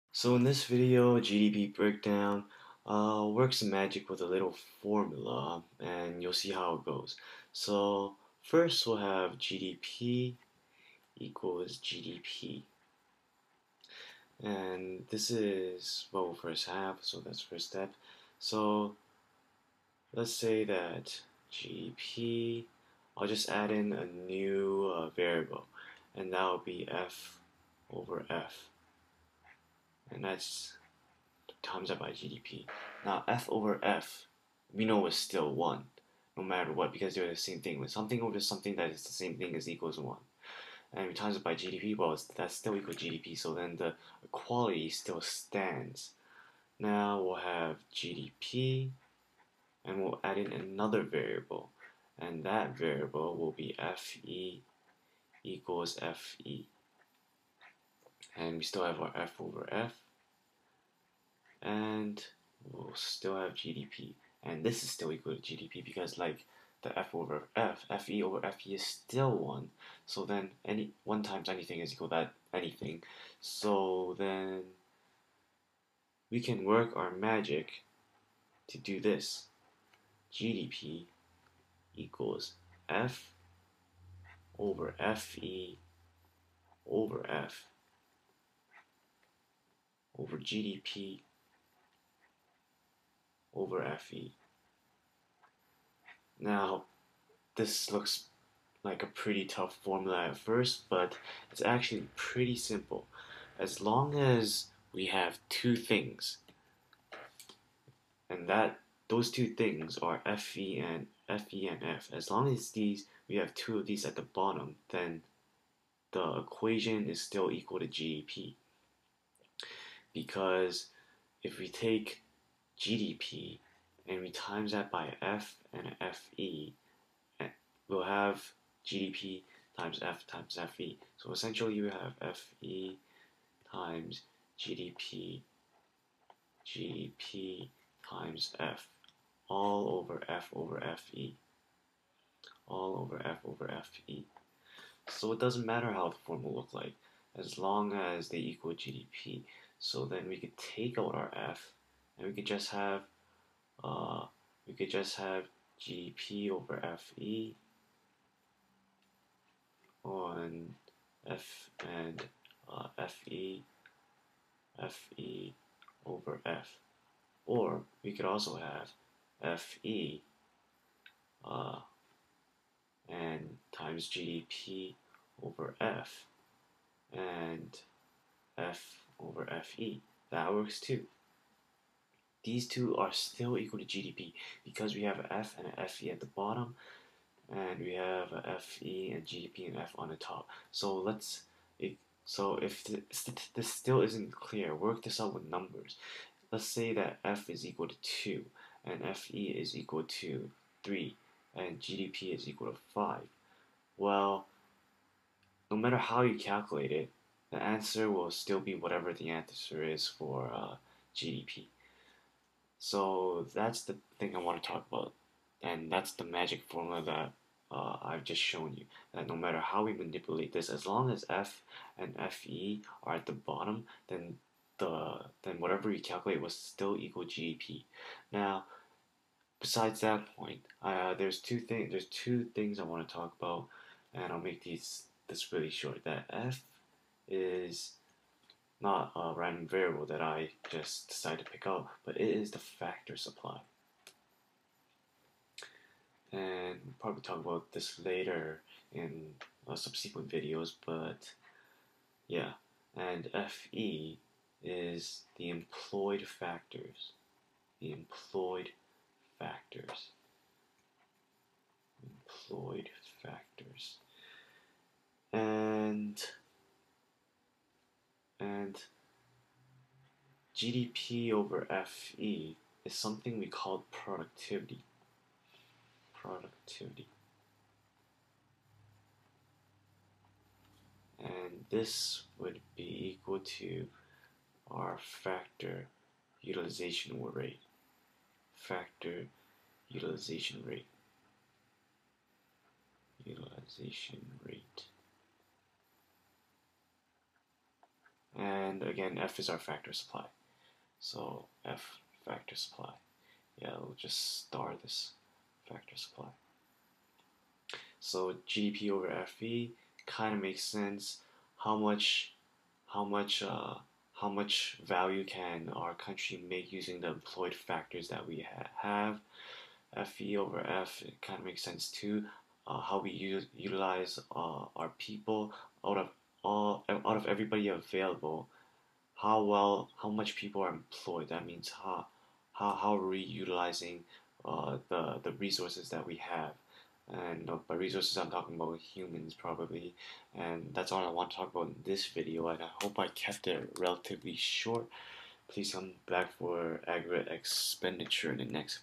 Macroeconomics - 9: GDP Breakdown
Coverage:

GDP
Factory supply
Employed factors
Factor utilization rate
Productivity